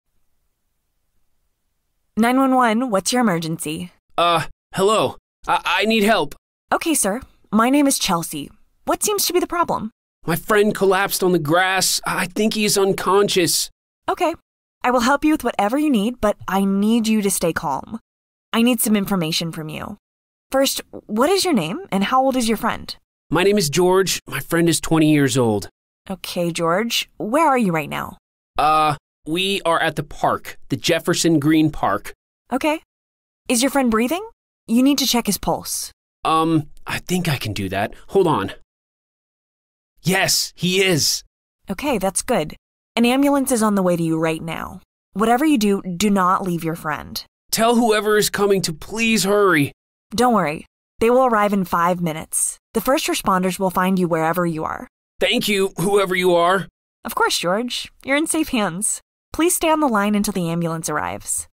AmeriLingua I 911, What’s Your Emergency ? I B2C1-1
911, what’s your emergency? In this ESL lesson, students will learn how to call 911 and identify possible reasons for calling in an emergency situation. Students will listen to a 911 call and discuss important things to do and remember in an emergency. Students will also learn and practice compound relative pronouns and learn and practice vocabulary and idioms relating to this topic. The lesson activities and worksheets have been developed for adult and teenage learners.